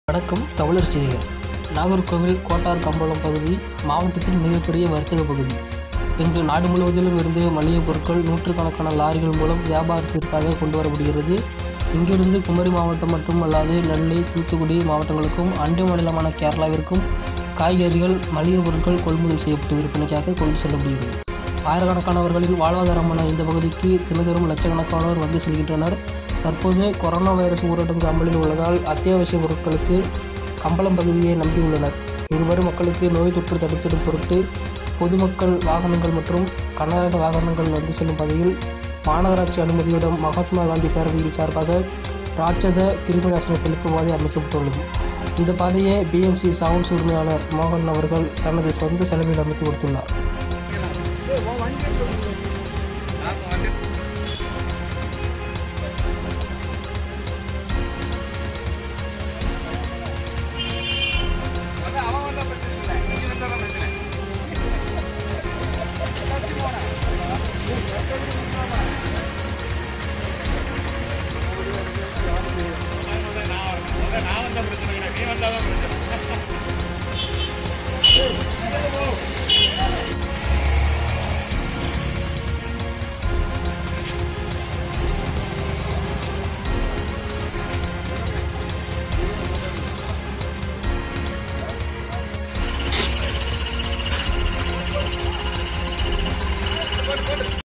நாகர்கோவில் கோட்டார் கம்போளம் பகுதி மாவட்டத்தின் மிகப்பெரிய வர்த்தக பகுதி இங்கு நாடு முழவதிலும்
நாகர்கோவில் கோட்டார் கம்போளம் பகுதி மாவட்டத்தின் மிகப்பெரிய  வர்த்தக பகுதி இங்கு நாடு முழவதிலும் இருந்து மளிகை பொருட்கள் நூற்றுக்கணக்கான  லாரிகள் மூலம் வியாபாரத்திற்காக கொண்டு வரப்படுகிறது இங்கிருந்து குமரி மாவட்டம் மட்டும் அல்லாது நெல்லை,தூத்துக்குடி மாவட்டங்களுக்கும் அண்டை மாநிலமான கேரளாவிற்கும் காய்கறிகள்,மளிகை பொருட்கள் கொள்முதல் செய்யப்பட்டு விற்பனைக்காக கொண்டு செல்லப்படுகிறது ஆயிரக்கணக்கானவர்களின் வாழ்வாதாரமான இந்தப் பகுதிக்கு தினந்தோறும் லட்சக்கணக்காணோர் வந்து செல்கின்றனர் தற்போது கொரானா வைரஸால் ஊரடங்கு அமலில் உள்ளதால் அத்தியாவசிய பொருட்களுக்கு கம்பளம் பகுதியையே நம்பி உள்ளனர் இங்கு வரும் மக்களுக்கு நோய் தொற்றை தடுத்திடும் பொருட்டு பொதுமக்கள் வாகனங்கள் மற்றும் கனரக வாகனங்கள் வந்து செல்லும் பாதையில் மாநகராட்சி அனுமதியுடன் மகாத்மா காந்தி பேரவை சார்பில் இராட்சத கிருமி நாசினி தெளிப்பு பாதை அமைக்கப்பட்டுள்ளது இந்த பாதையை பி.எம்.சி சவுண்ட்ஸ் உரிமையாளர் மோகன் அவர்கள் தனது சொந்த செலவில் அமைத்து கொடுத்துள்ளார்